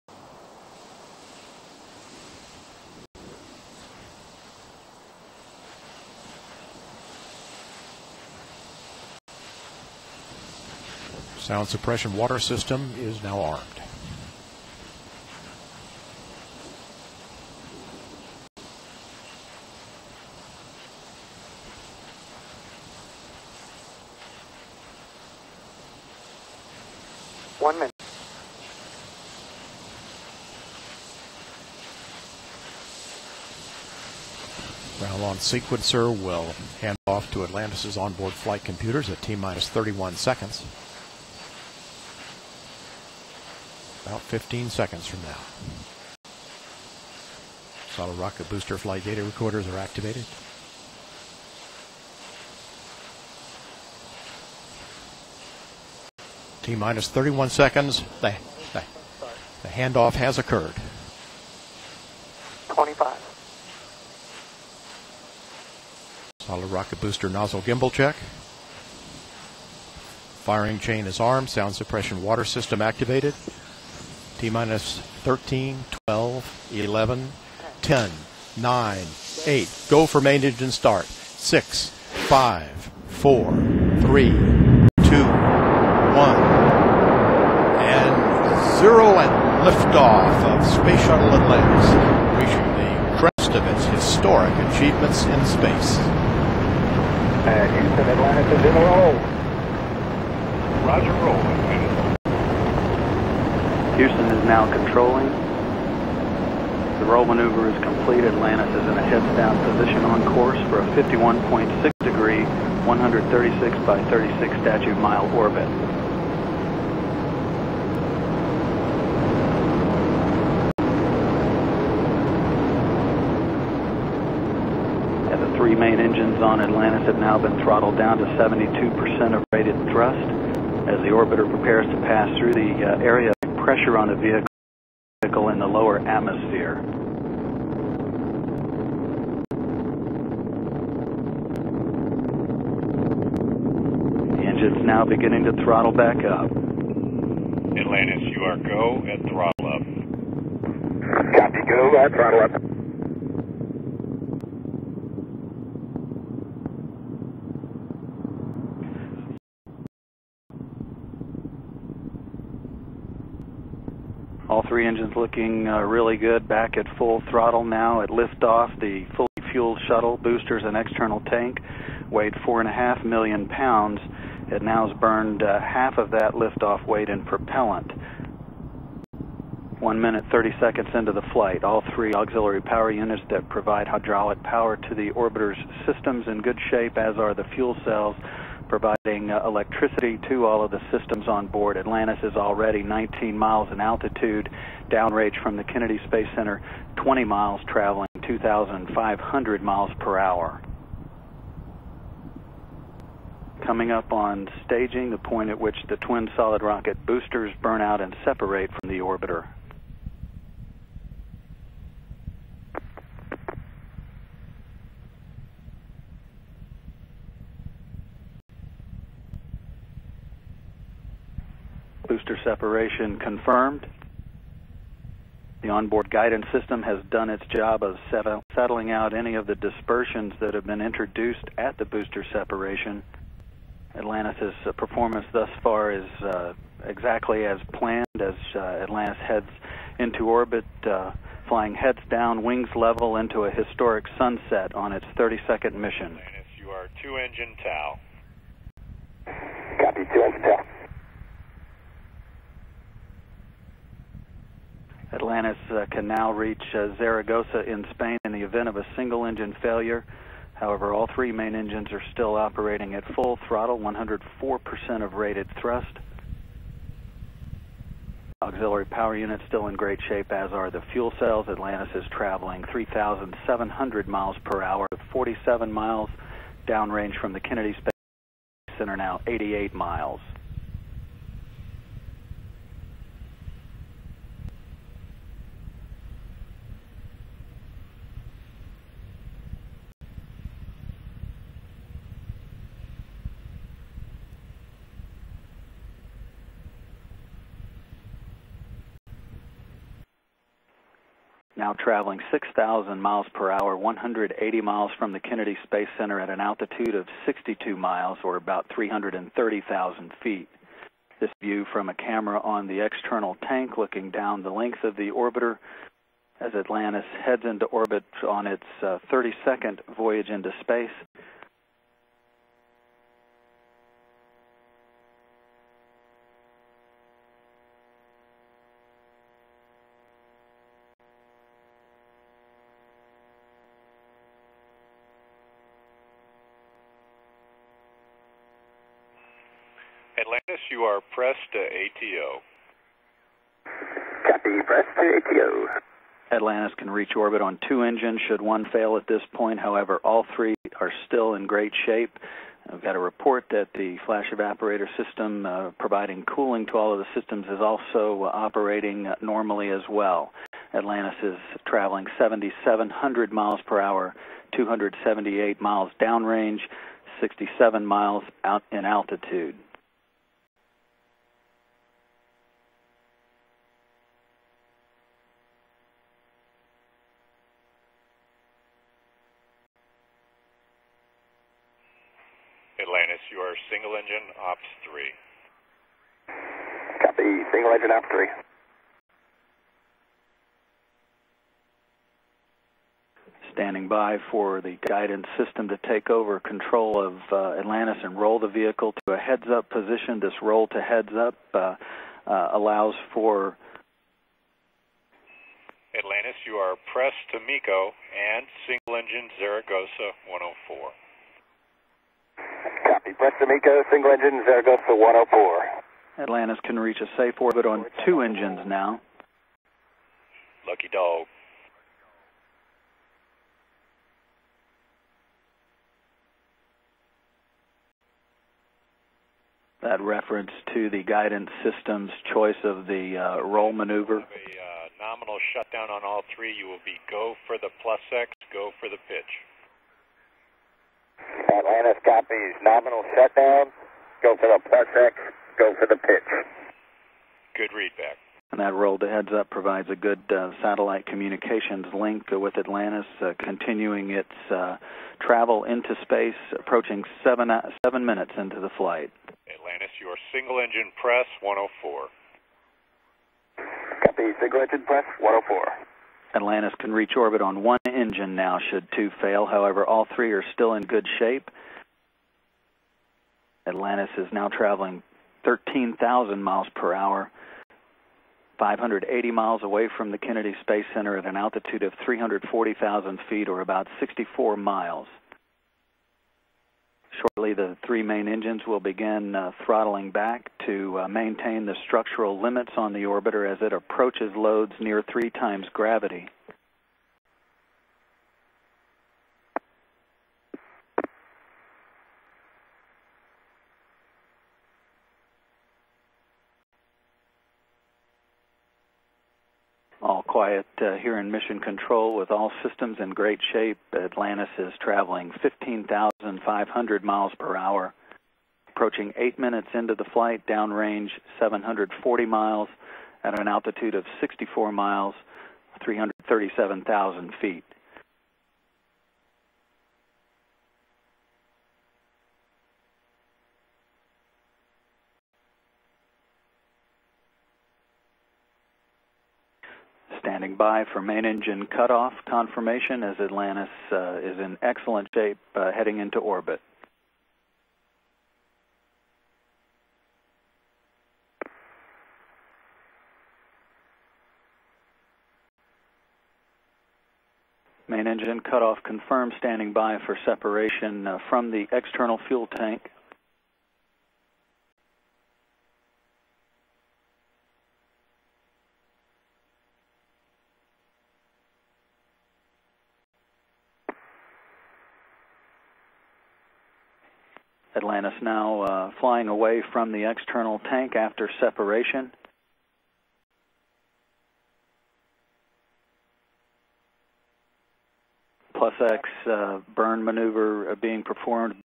Space Shuttle Atlantis STS-132 Launch (+9 MECO) SD
Courtesy: NASA TV

Standard Definition. Will upload HD later...